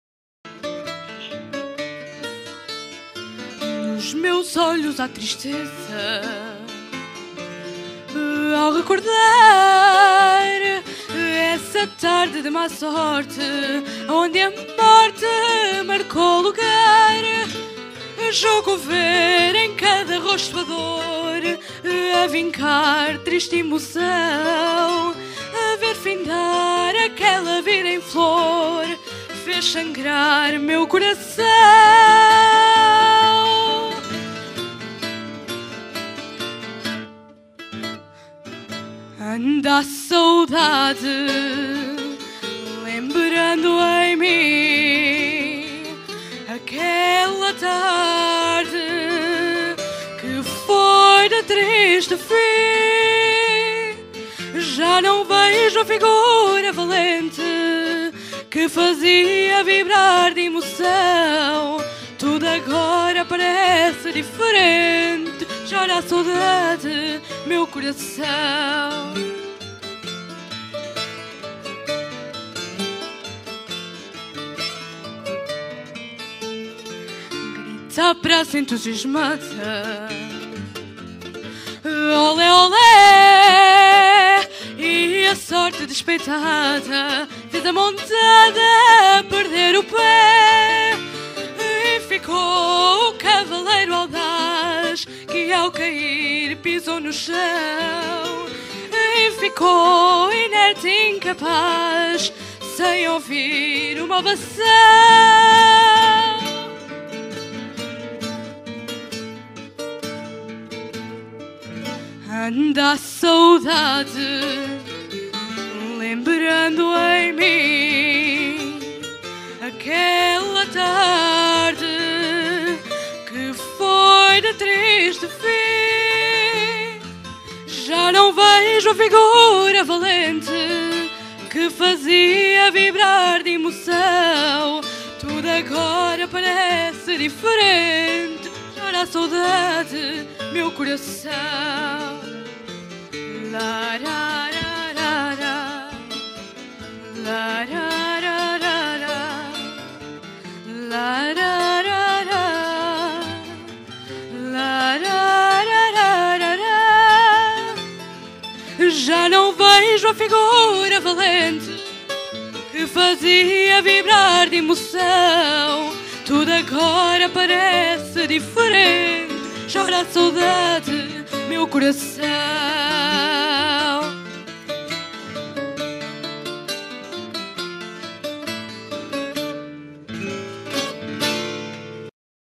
Maria Teresa Casaca no Concurso de Fados do Cartaxo
Maria Teresa Casaca na 1ª eliminatória do I Concurso de Fado Amador do Concelho do Cartaxo, que decorreu em Vale da Pinta em 25 de Fevereiro de 2012. Tal como os restantes candidatos foi acompanhada por Marco Oliveira (viola) e Guilherme Banza (guitarra).